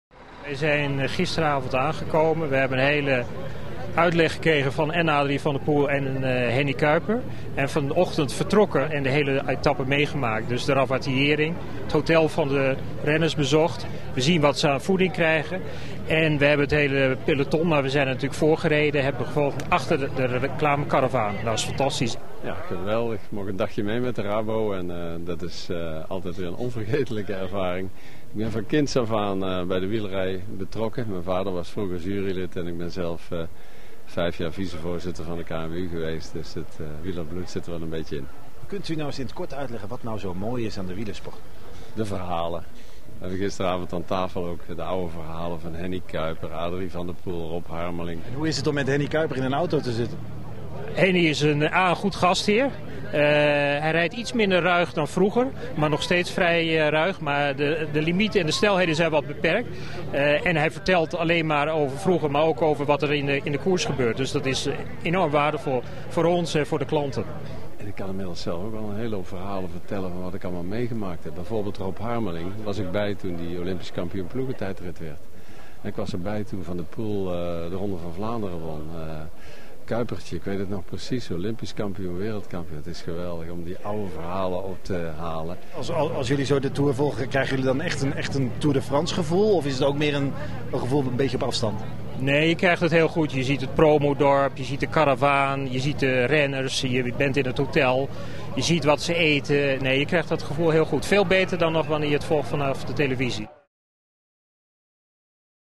Op bezoek in de Tour
De Tour trekt vele hoogwaardigheidsbekleders aan waaronder burgemeester Rombouts van Den Bosch en Sipko Schat, lid van de Raad van Bestuur van Rabobank Nederland.